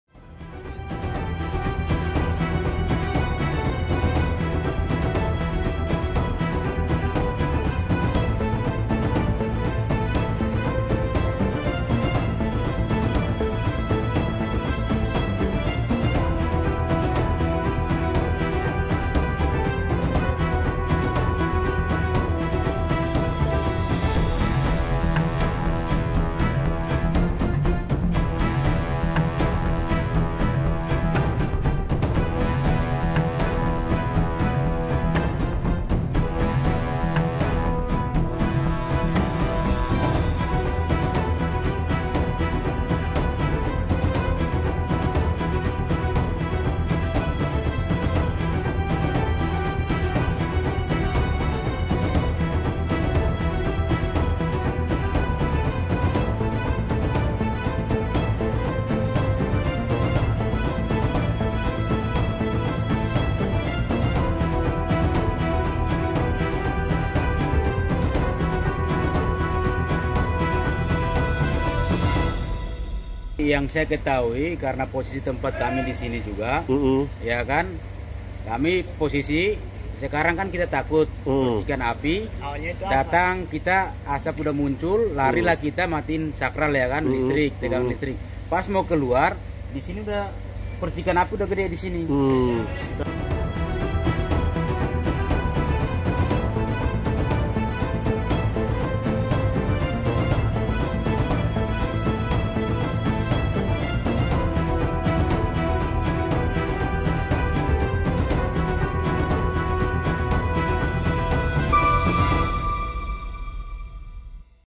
Kantor Pajak Tebet Terbakar, 10 Mobil Damkar Diterjunkan
Kantor Pajak di kawasan Tebet, Jakarta Selatan terbakar, Jumat (28/5) subuh. Api berkobar dan dengan cepat melumat habis kantor beserta isinya. Warga dan keamanan gedung sempat panik. Warga takut  api menyambar rumah mereka.

Api diketahui muncul pertama kali di lantai tiga. Dugaan sementara, api berasal dari hubungan arus pendek listrik. Sepuluh unit damkar dikerahkan ke lokasi, video selengkapnya di